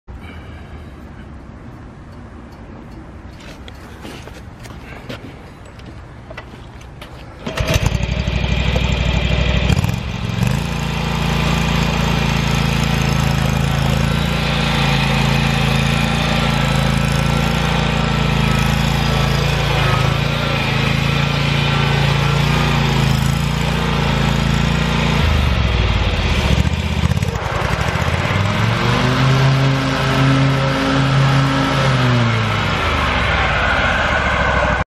Hustler Trimstar 930396 lawn mower for sale at auction | bidding closes February 27, 2019
Hustler Trimstar 930396 lawn mower
1,741 hours on meter
48" cut
Kawasaki two cylinder gas engine
15 HP
Hydrostatic transmission
Velky sulky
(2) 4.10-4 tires
11x4.00-5 front tires
20x10.00-10 rear tires
Serial 12090737
Unit# 3
It's located in Saint Charles, MO, and we're selling it to the highest bidder regardless of price.